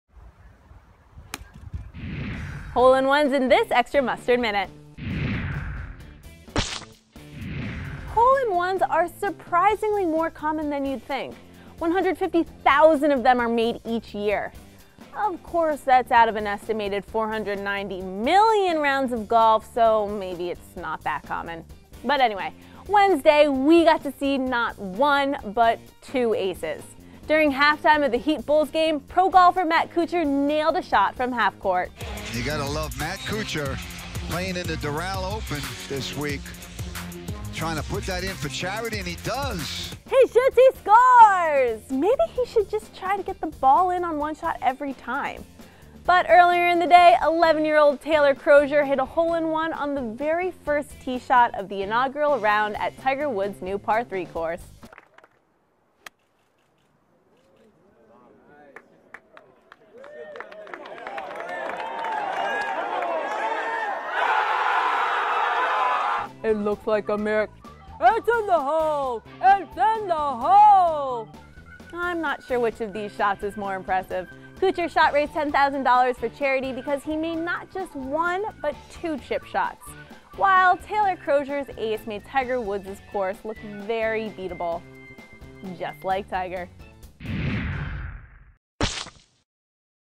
Whose hole-in-one was better? Matt Kuchar or this 11-year old | Sports Illustrated
Two hole-in-ones were shot on Wednesday: One by professional golfer Matt Kuchar, the other by 11-year old junior PGA golfer Taylor Crozier.